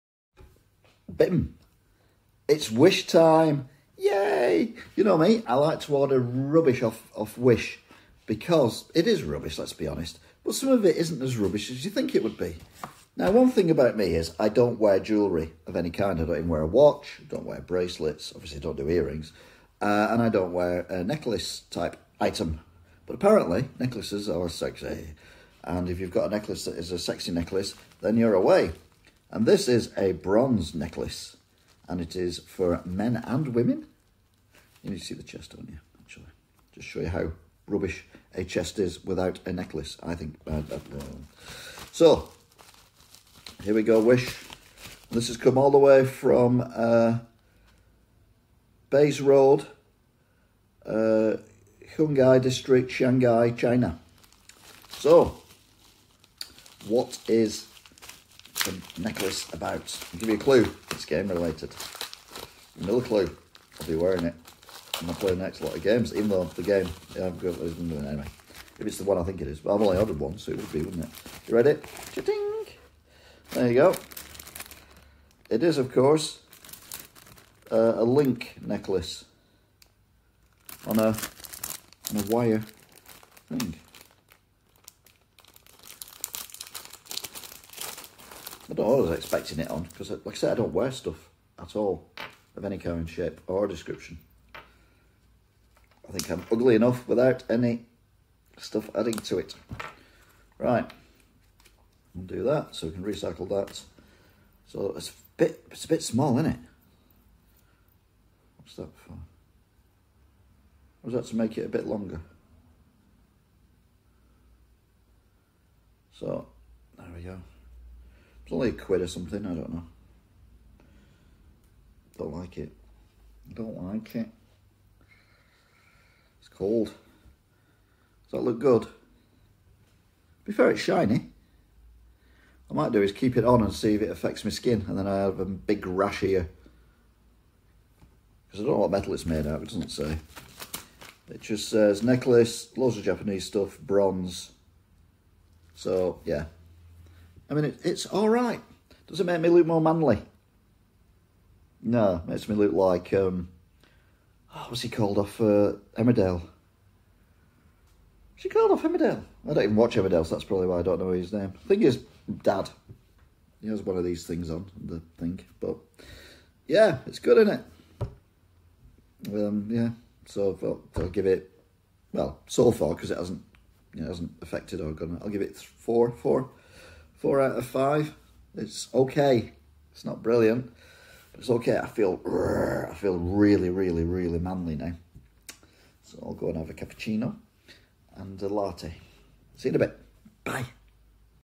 Wish.com review Link Necklace
we all love Zelda, well you do. but a wish Link Necklace, gotta be great hasn't it??